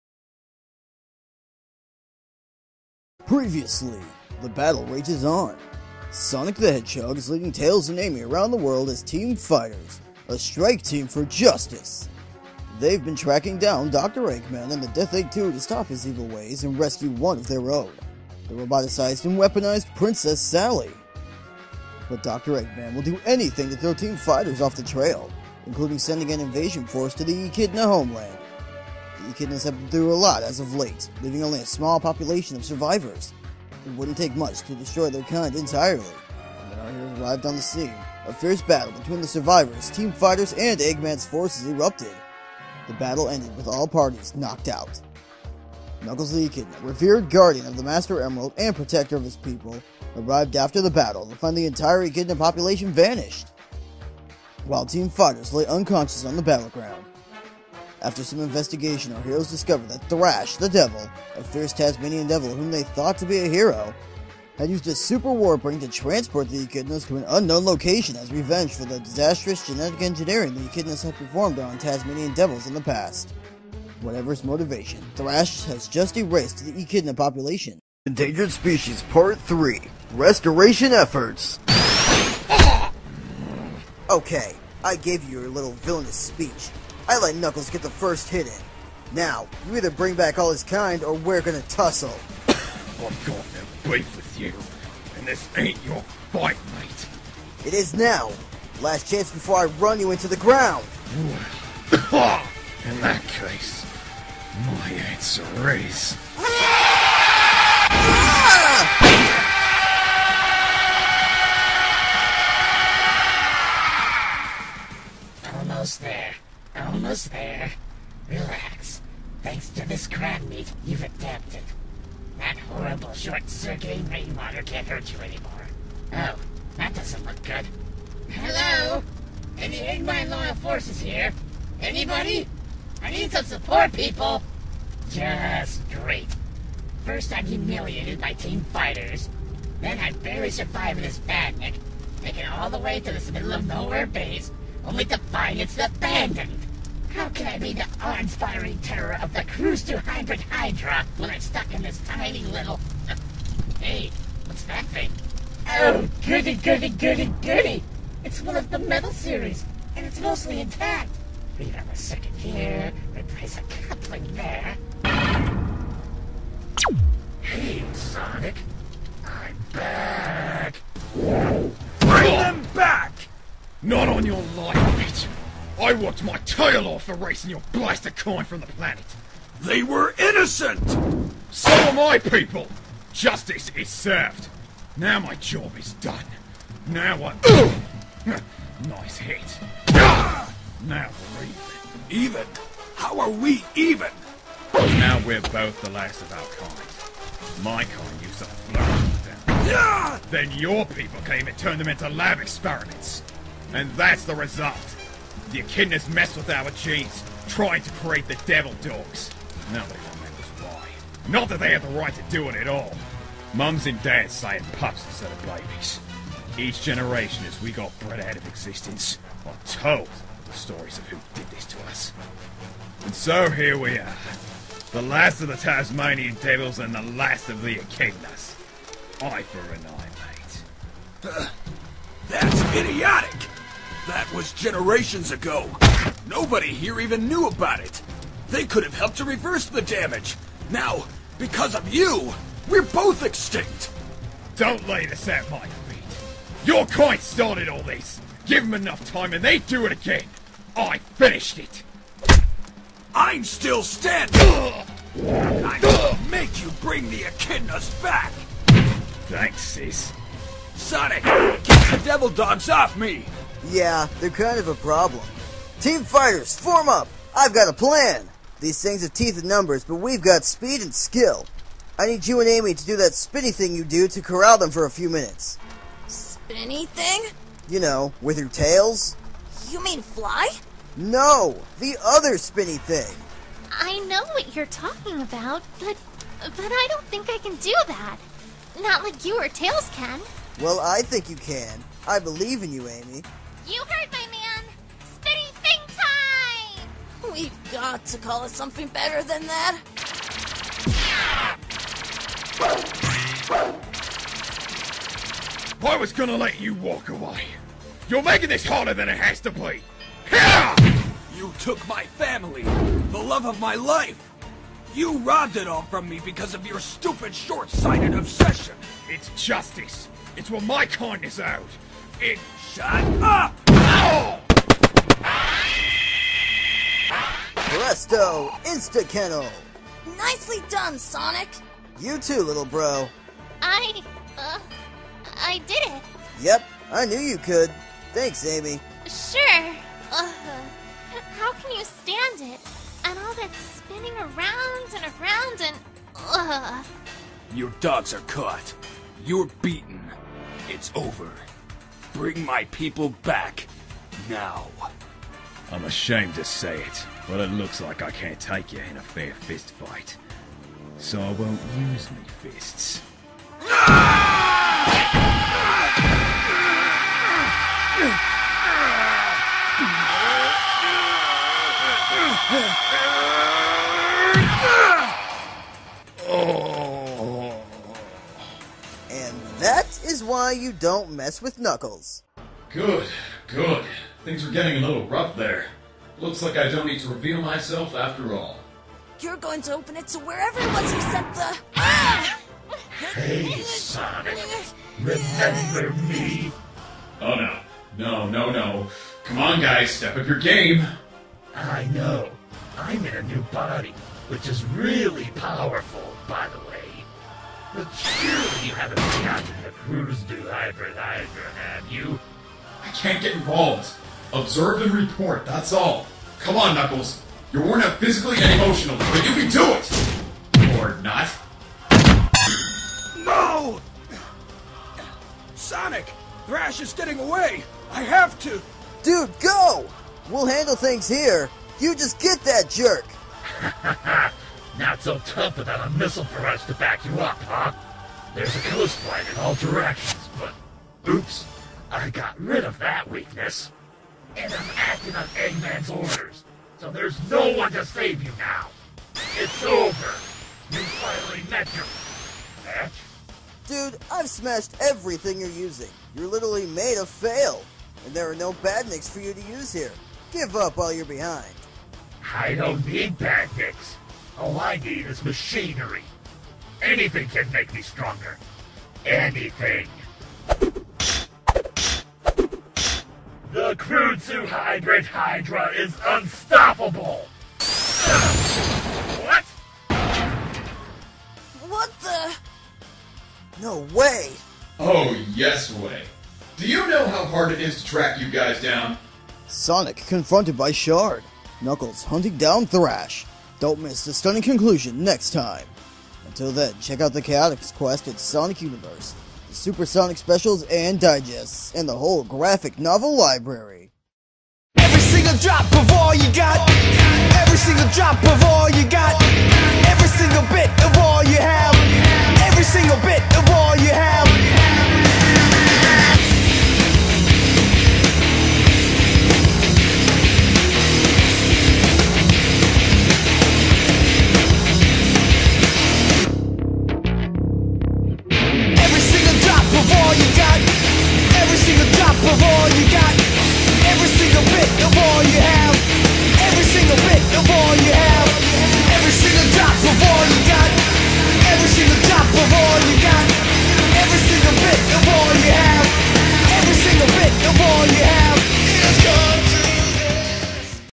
Sonic The Hedgehog #245 Endangered Species Part 3 Comic Drama
Original upload Aug 14, 2015

It's the final part to endangered species!
Will Knuckles save his family and friends or is he stuck all by himself?

Category